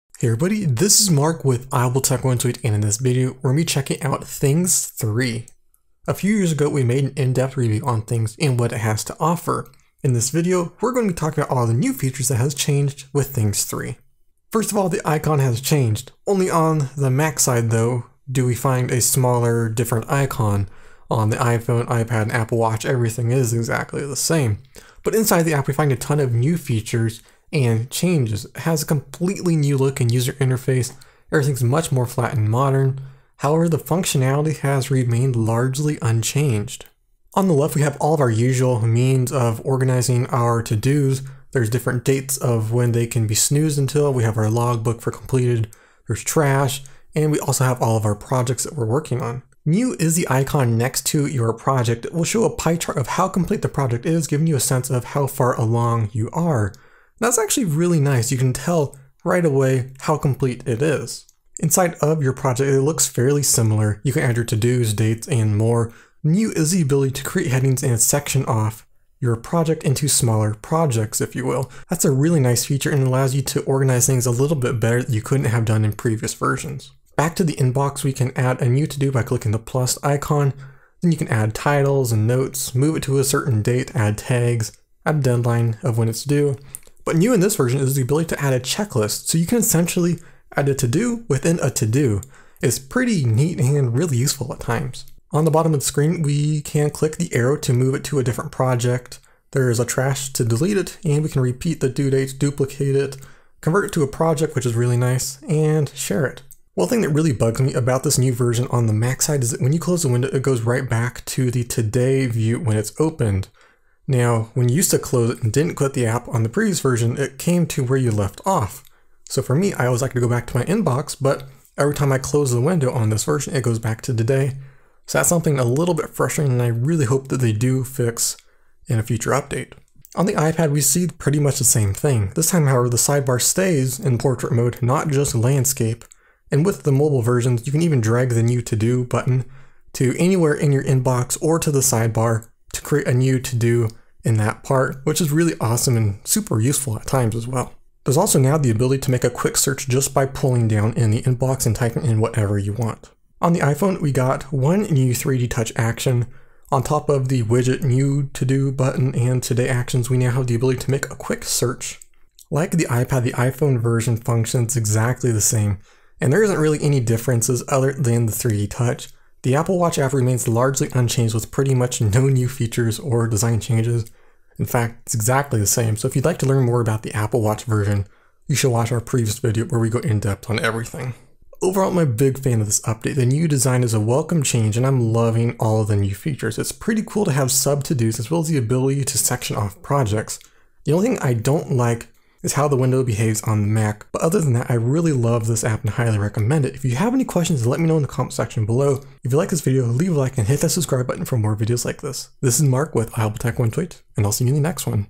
Things 3 Review
In this video we take a look at Things 3 and all of the new features it has to offer. We take about everything that’s new and why you should upgrade.

Send us stuff:
Mark Paris